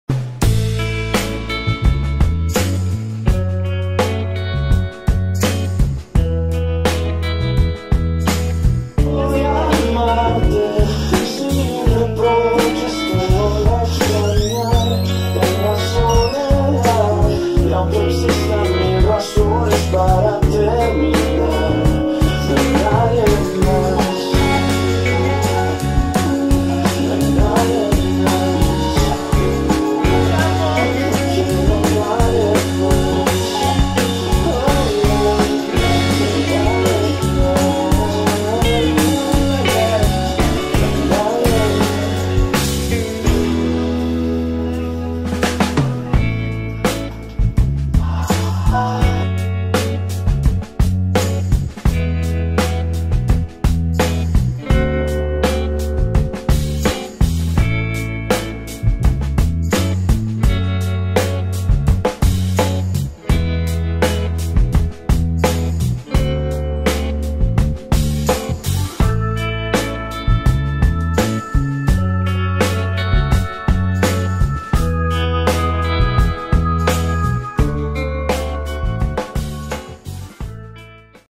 La emoción del parto.
Cuándo decides hacer un parto en agua y en casa. La emoción crece al máximo.